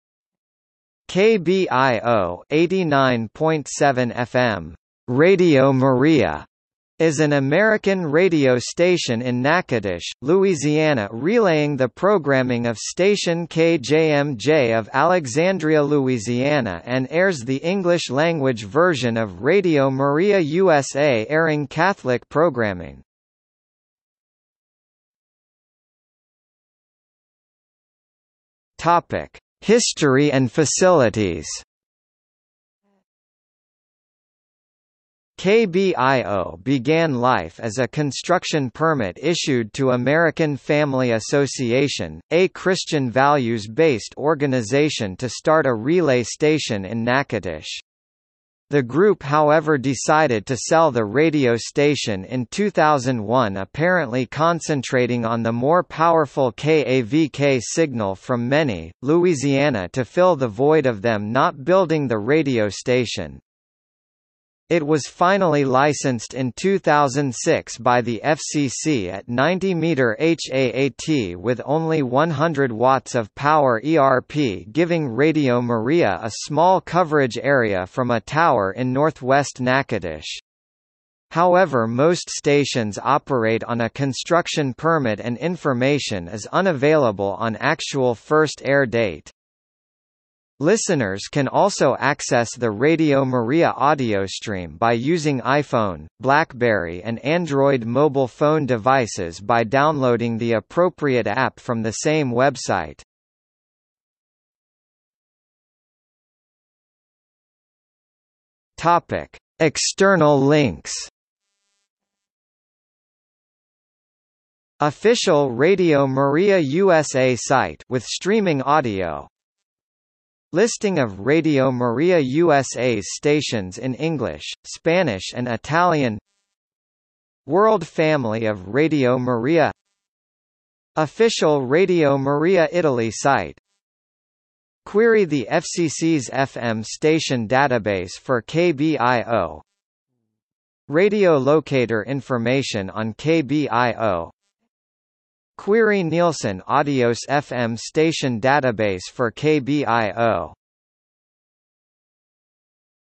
KBIO | Wikipedia audio article
00:00:25 History and Facilities
00:01:45 External links

- Socrates

SUMMARY
KBIO (89.7 FM, "Radio Maria") is an American radio station in Natchitoches, Louisiana relaying the programming of station KJMJ of Alexandria, Louisiana and airs the English-language version of Radio Maria USA airing Catholic programming.